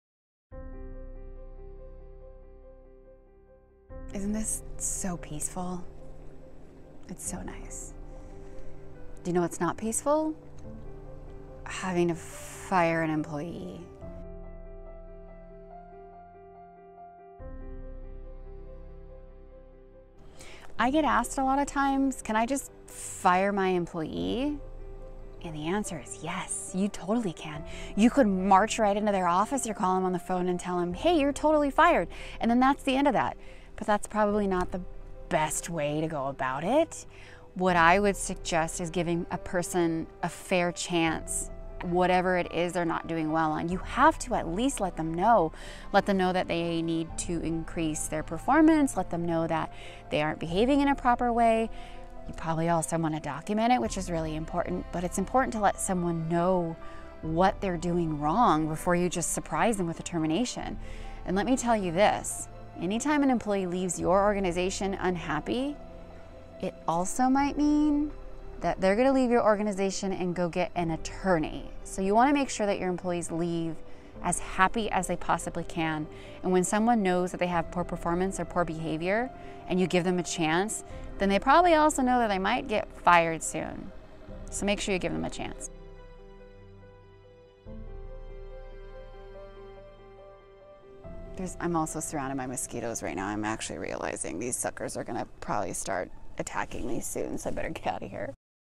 Can I fire my employees? - Asking For A Friend
As an employer or even a manager, there may come a time when you have to have "the talk" with one or more of your employees. No not "THAT TALK"... This is more of a "Clear your desk and good luck on your future endeavors" type of conversation.

The question you may be having is "can I fire my employees"? The simple answer is yes, you can. You can literally march up to your employee and fire them on the spot if you wanted to (we're talking at-will employment here). However, we wouldn't recommend that approach unless you're testing out how great of an attorney you have. Haha, we're kidding!

Stop and ask, how did the situation get to this point? Did you just wake up and think "Wow, I really want to fire Joe today" or was there more to this?
Our suggestion would be to have a talk with your employee first and see if the situation improves. ✨

In this episode of Asking For A Friend, we answer the question, "can I fire my employees" and we share our thoughts on how to give a proper goodbye to employees.

If your business needs some of our lighthearted and fun Human Resources  Support, we can help!